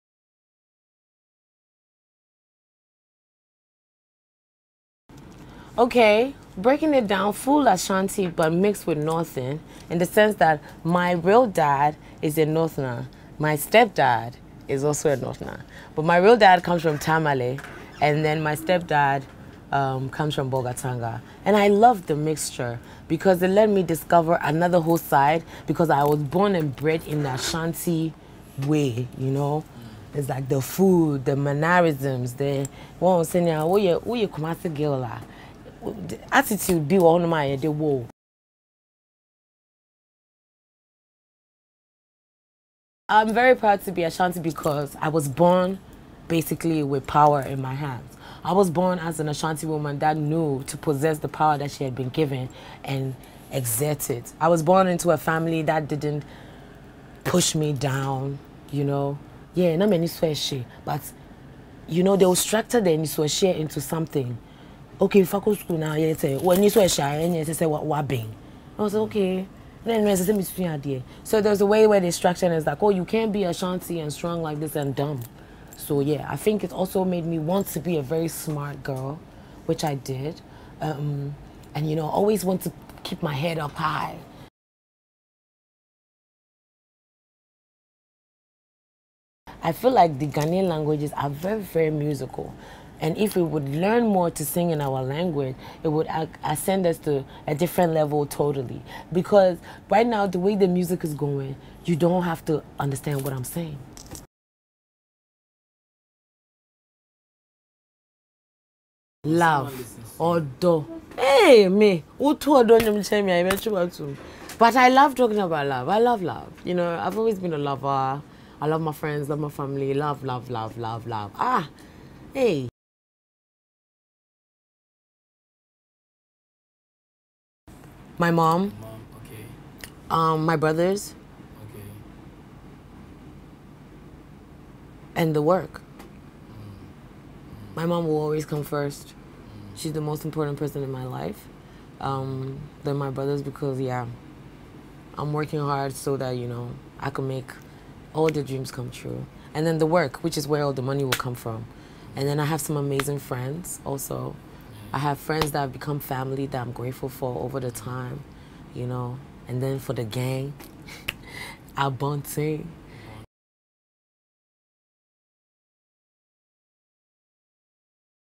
Efya | The Culture , The Sound & The Person | Afrochella Music Museum 2021
Get to know what inspires @efyamusic's music and how being Ashanti gives her the strength to keep going.

Learn more Afrochella.com